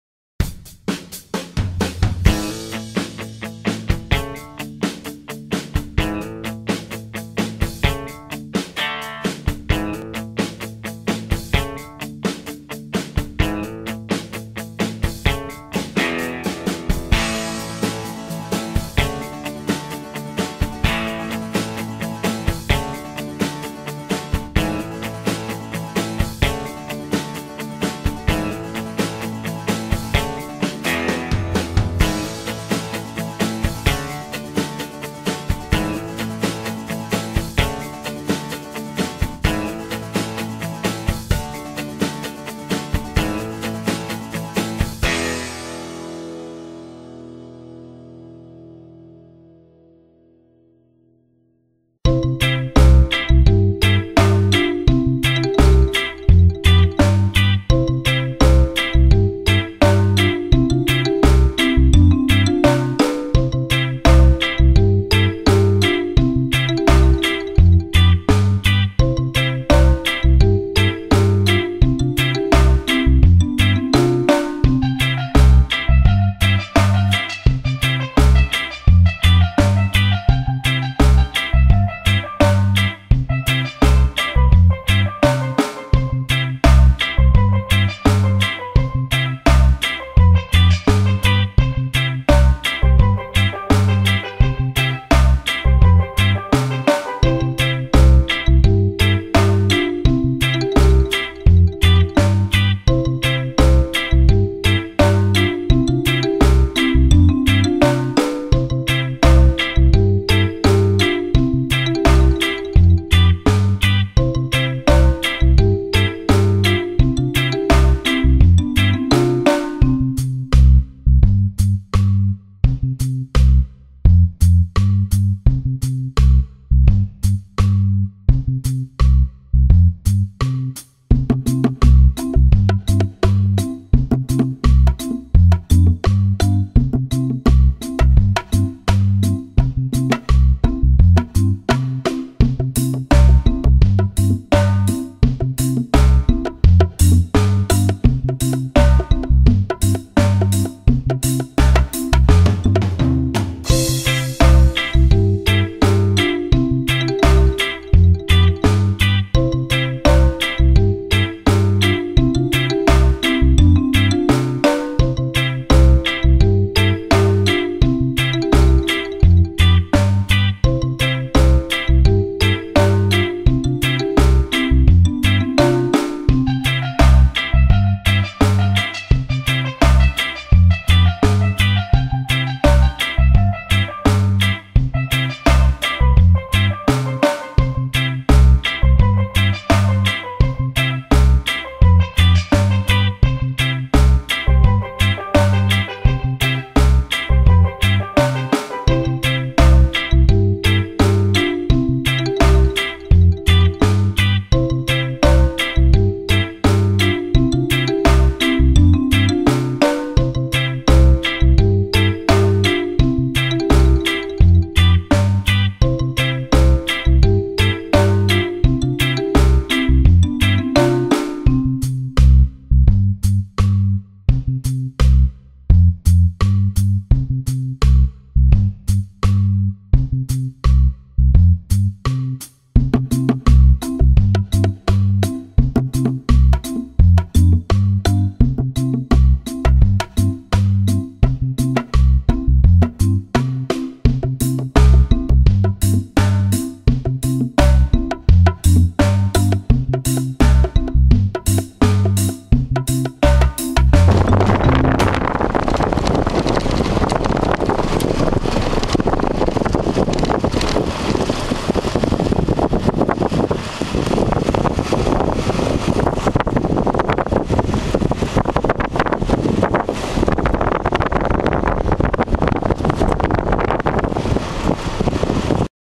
Boat Harbour Kurnell
Cruising the beach at Boat Harbour 4X4 Park at Kurnell, Sydney. I made this vid to show those what Boat Harbour is about so they can decide whether they want to pay the $25 entry as there is little information or pics on the net to help you decide. The 4X4 part is actually closed now, as it has been mined of all it's sand. Access is only available on the beach. The track is very rough on the road in/out so the camera gets a little shaky (sorry).